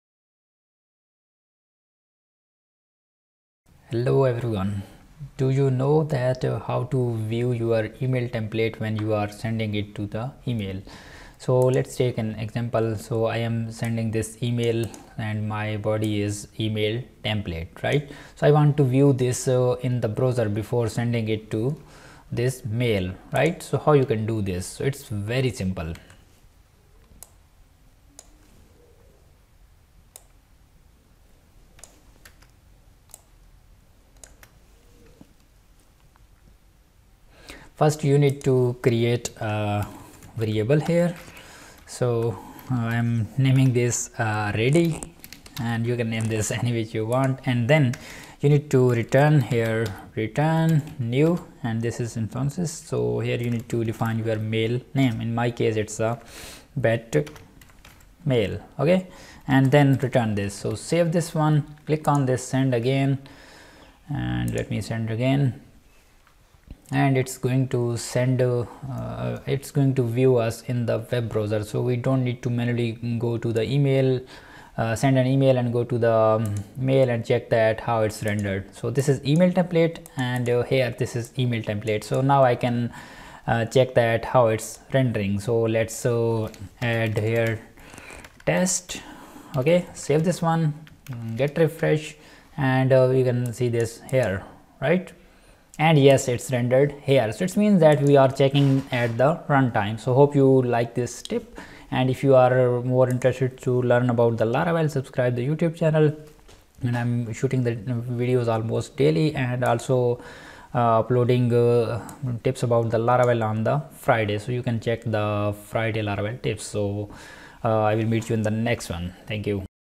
Laravel Email | View Email Template in browser | View email design before sending it through email
Before sending email view it's design in web browser.
print email template before sending it.

How to view design of email before sending it.

Laravel full series
Laravel scratch to advance series
Laravel zero to hero course
Laravel guide

The project is stored in the given below directory.